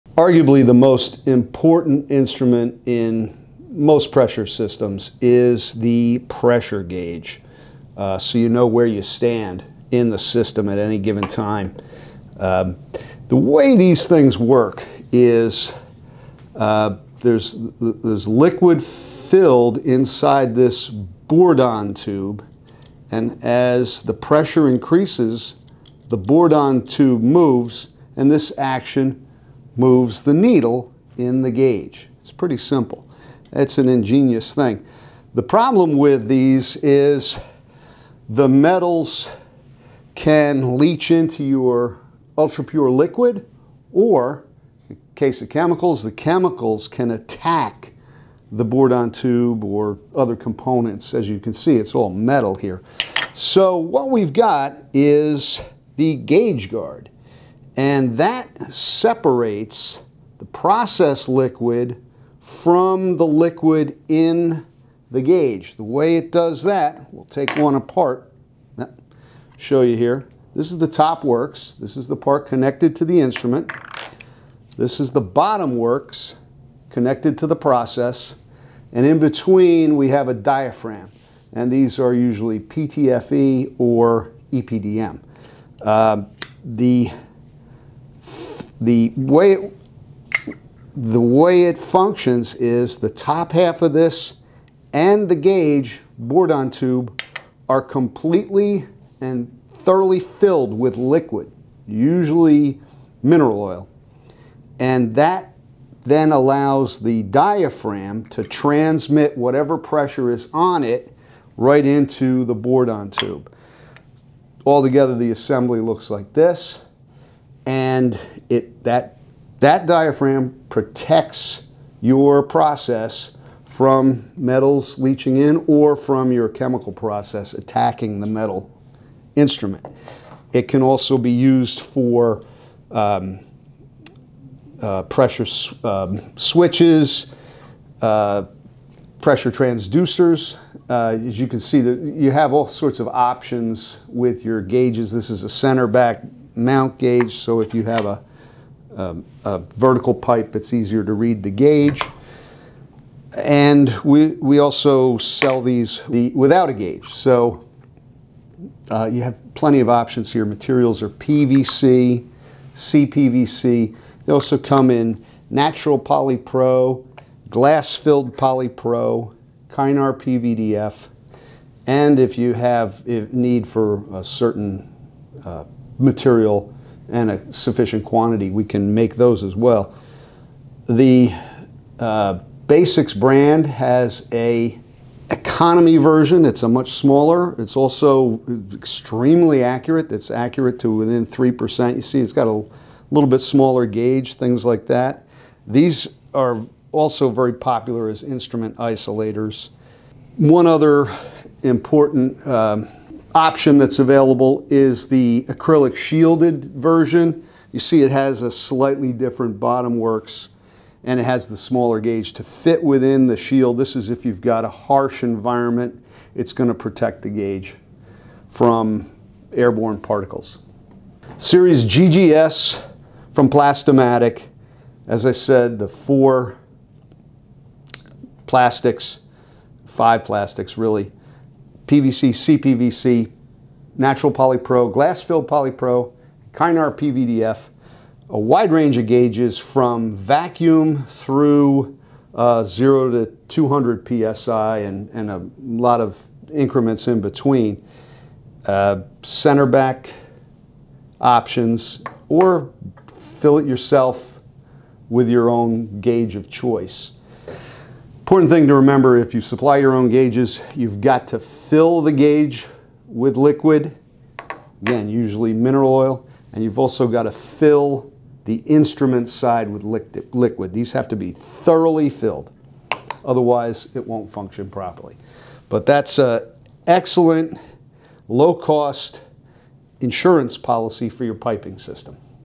What, How, Where & Why of Gauge Guards
Plast-O-Matic Advertising Manager Rick Bolger provides an intro to pressure gauges, Bourdon tubes, diaphragm seals, and describes the role of a Gauge Guard, AKA Instrument Isolator, in a corrosive or ultrapure liquid piping system